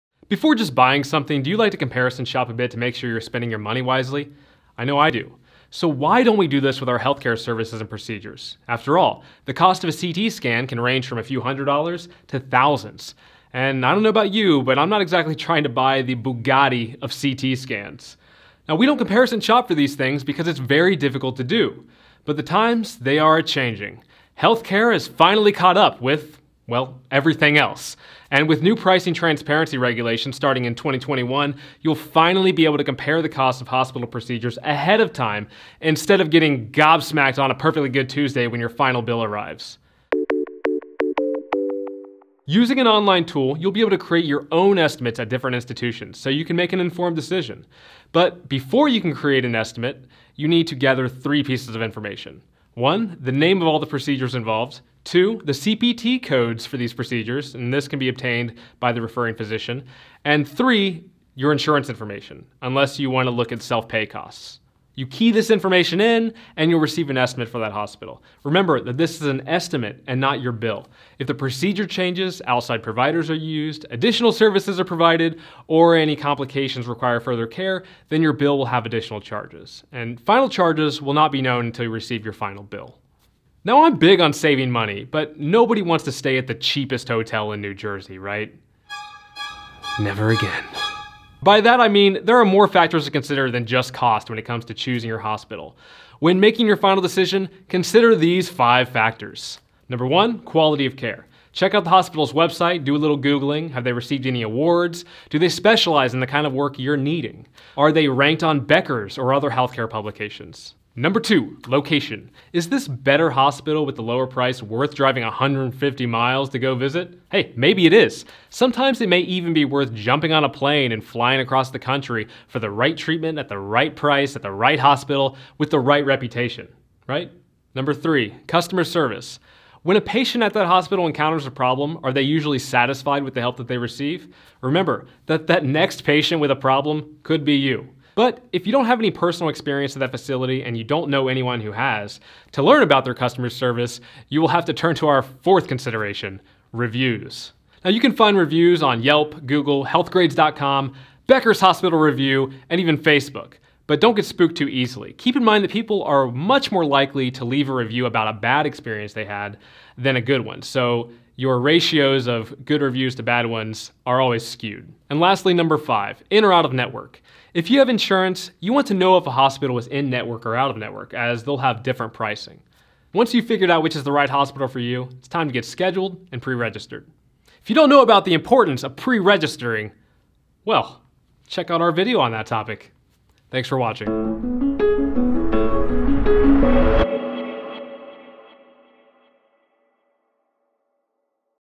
What is Price Transparency? | New Regulation Changes Explained
What is price transparency? In 2020, new regulation was passed that would require hospitals to disclose their prices. What does price transparency mean for you? This lets you make more informed financial decisions when it comes to your healthcare services.